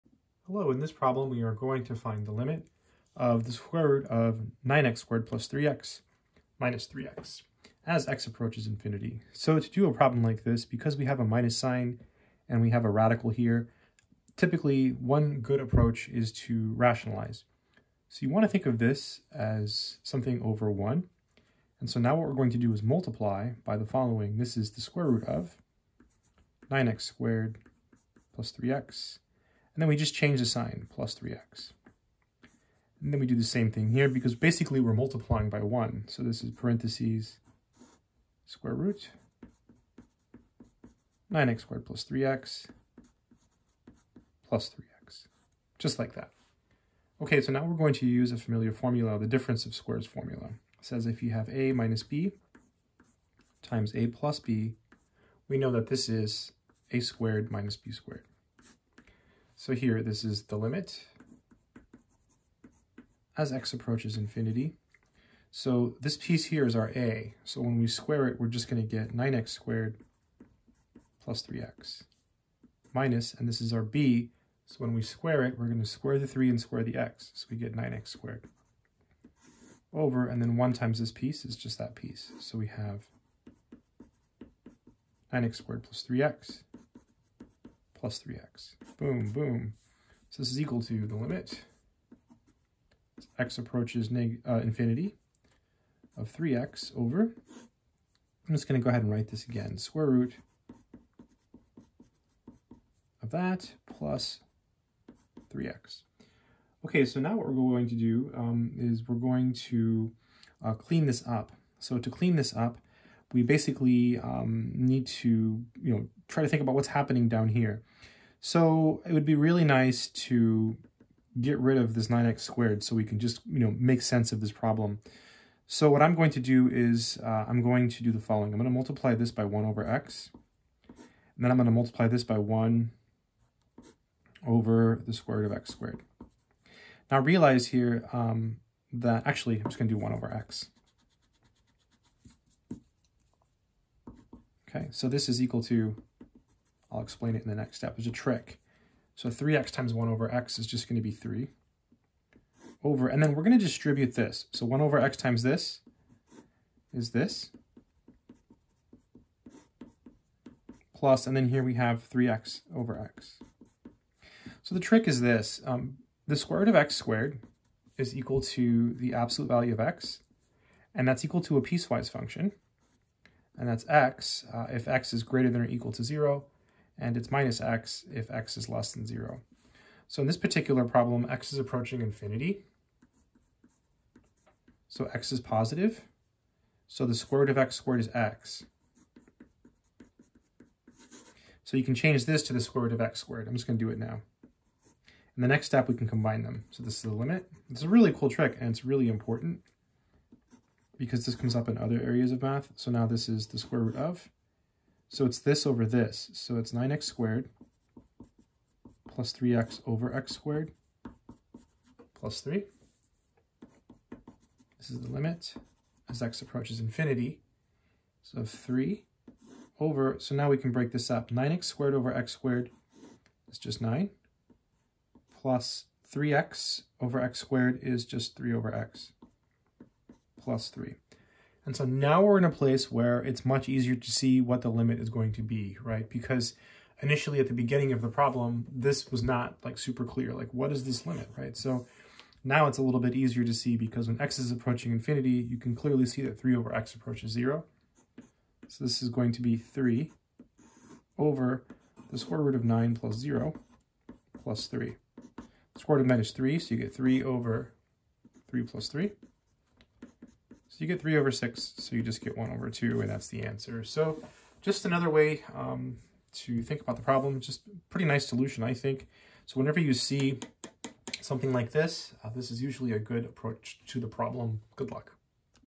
Limit of sqrt(9x^2 + 3x) - 3x as x approaches infinity
Limit of sqrt(9x^2 + 3x) - 3x as x approaches infinity. In this video we do a calculus problem where we find a limit. This one is harder and we rationalize the numerator at the beginning of the problem.